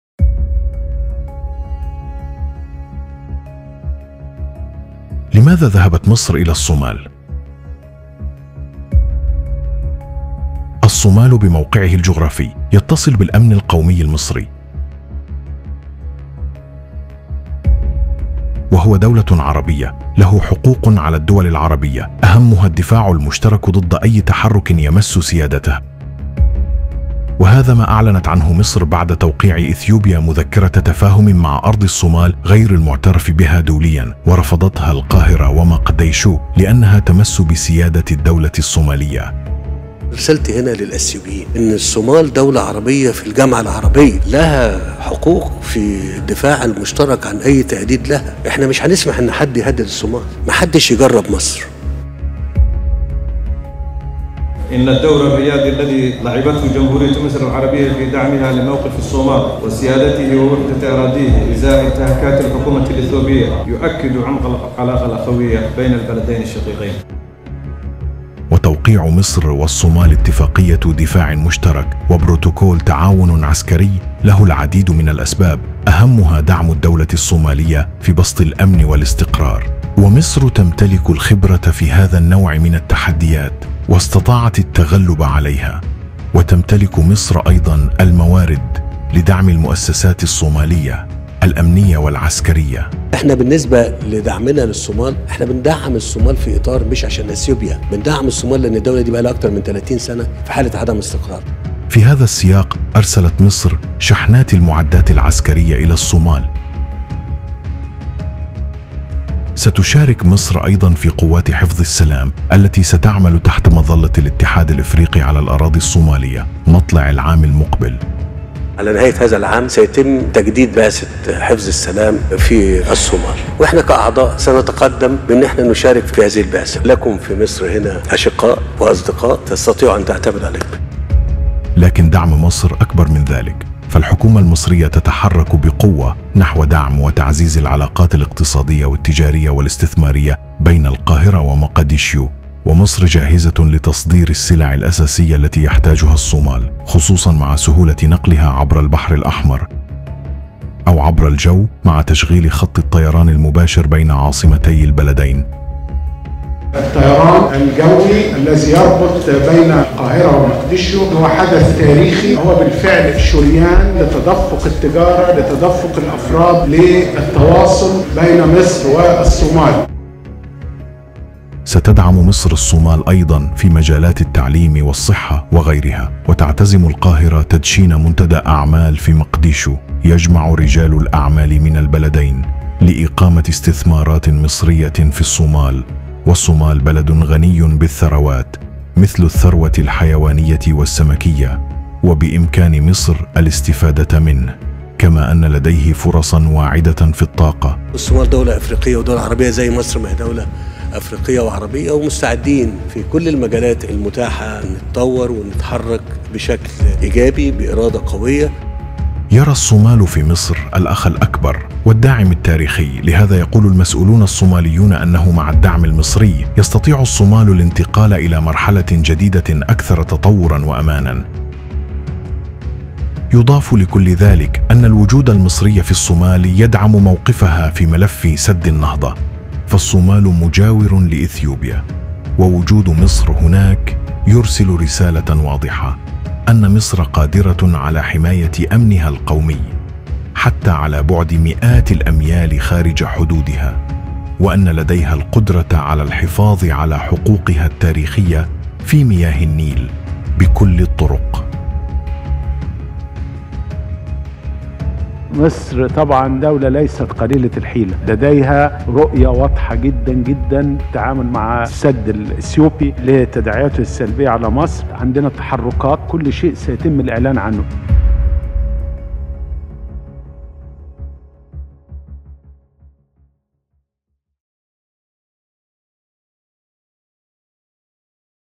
ماعت جروب | لماذا أرسلت مصر أسطولها ومعدات الجيش إلى الصومال؟ خفايا رسائل الردع وسر تحرك غير مسبوق
لماذا ذهبت مصر إلى الصومال؟
الصومال بموقعه الجغرافي، يتصل بالأمن القومي المصري.
وهي دولة عربية.. له حقوق على الدول العربية، أهمها الدفاع المشترك ضد أي تحرك يمس سيادتها.
وهذا ما أعلنت عنه مصر بعد توقيع إثيوبيا مذكرة تفاهم مع أرض الصومال غير المعترف بها دوليا، ورفضتها مصر والصومال، لأنها تمس بسيادة الدولة الصومالية.
وتوقيع مصر والصومال اتفاقية دفاع مشترك، وبروتوكول تعاون عسكري، له العديد من الأسباب..
أهمها دعم الدولة الصومالية في بسط الأمن والاستقرار.. ومصر تملك الخبرة في هذا النوع من التحديات، واستطاعت التغلب عليها.
وتملك مصر أيضا الموارد.. لدعم المؤسسات الصومالية، الأمنية والعسكرية.
في هذا السياق أرسلت مصر شحنات المعدات العسكرية إلى الصومال.
ستشارك مصر أيضا في قوات حفظ السلام التي ستعمل تحت مظلة الاتحاد الإفريقي على الأراضي الصومالية، مطلع العام المقبل.